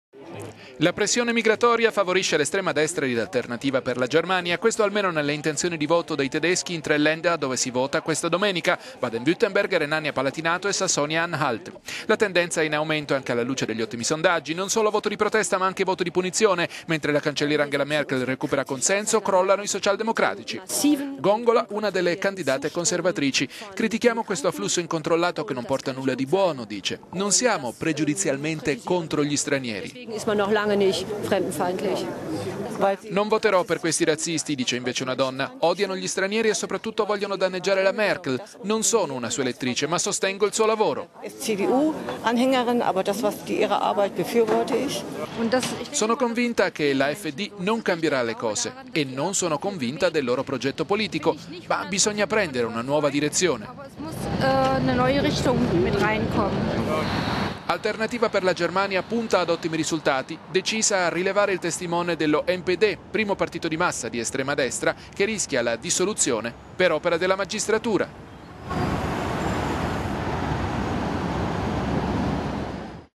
Pressione migratoria favorisce estrema destra in Germania
La pressione migratoria favorisce l'estrema destra di Alternativa per la Germania. Questo almeno nelle intenzioni di voto dei tedeschi in tre länder dove si vota questa domenica: Baden Würtenberg, Renania Palatinato e Sassonia Anhalt.

La tendenza è in aumento anche alla luce degli ottimi sondaggi. Non solo voto di protesta, ma anche voto di punizione. Mentre la cancelliera Angela Merkel recupera consenso, crollano i socialdemocratici nelle intenzioni di voto.